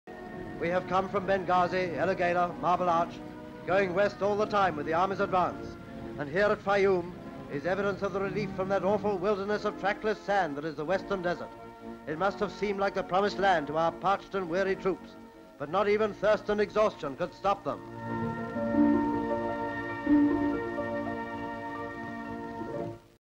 Photos of oasis in North Africa (1943)
Scenes from the oasis within the desert west of Benghazi

Full Description:

AFRICA: Western Desert:
EXT

Scenes from the oasis within the desert west of Benghazi, shows military transport vehicles on desert road, shows local residents herding animals

WWII, World War II, Second World War, Axis, Allies, transport trucks, trucking , lorry, lorries
Background: Scenes from the oasis within the desert west of Benghazi